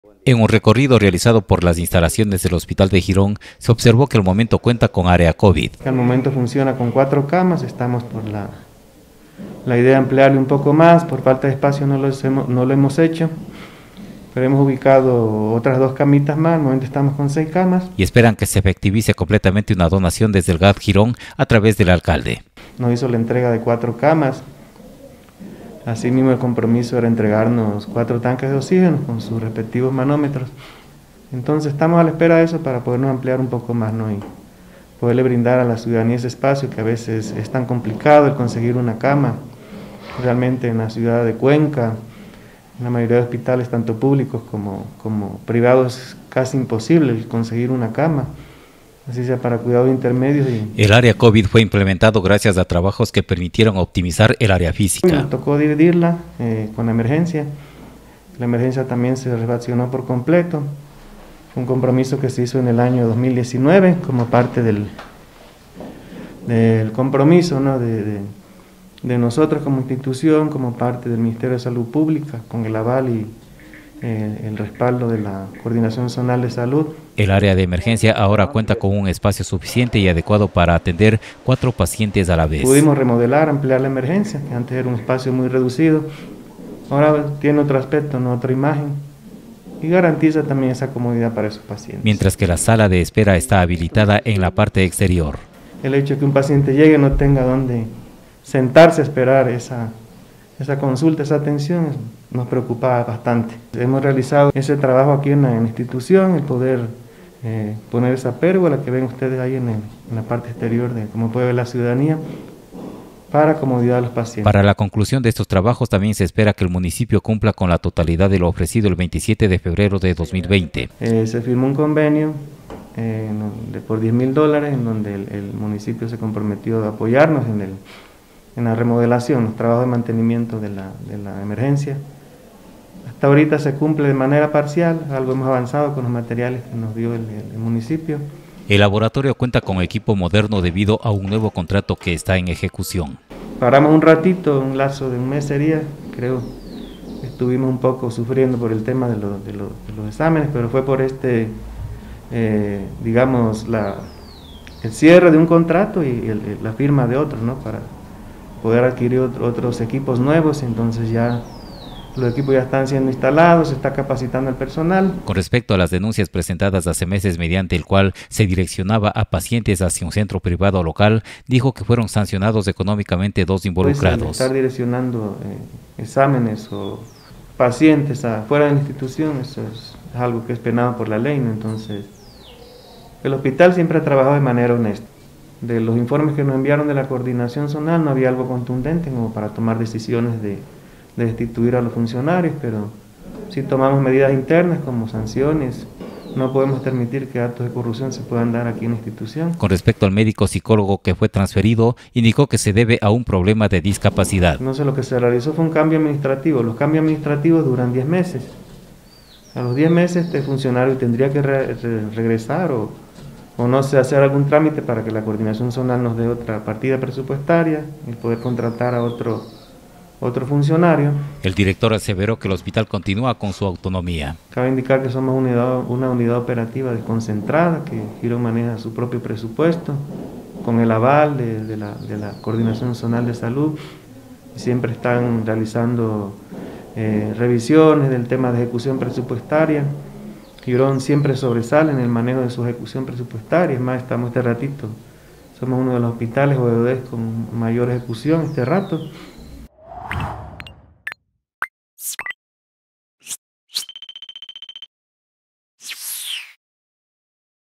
Hospital de Girón registra avances informa el director de la casa de salud
Luego de un largo silencio ordenado desde instancias superiores, hemos podido dialogar con el director del hospital Aida León de Rodríguez Lara del cantón Girón, el doctor Alex Torres, el galeno dio a conocer avances de la institución de salud en los últimos meses, como también los requerimientos que urgen. Se refirió además a la denuncia planteada en contra de funcionarios que habrían direccionando pacientes hacia un local particular de atención médica; y, reitero que la casa de salud gironense se mantiene con su categoría.